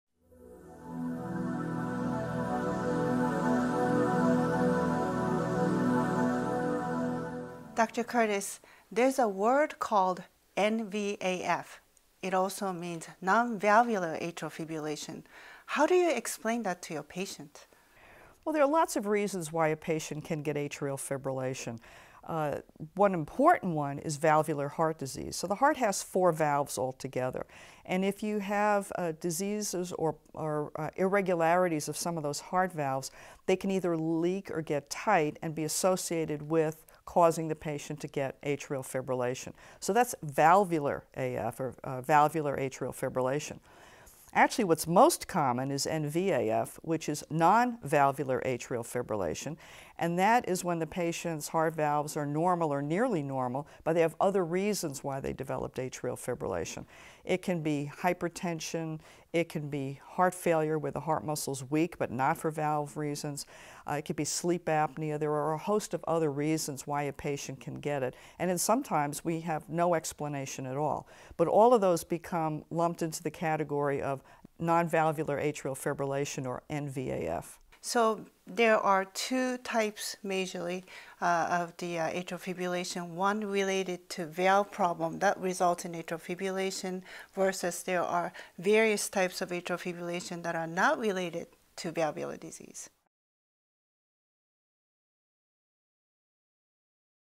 What is non-valvular atrial fibrillation (NVAF)?
In this video, cardiologists Dr. Anne Curtis and Dr. Hiroko Beck explain what non-valvular atrial fibrillation (NVAF) is compared to valvular atrial fibrillation. Non-valvular atrial fibrillation can be caused by non-valvular reasons such as hypertension, heart failure, sleep apnea, or for unknown reasons.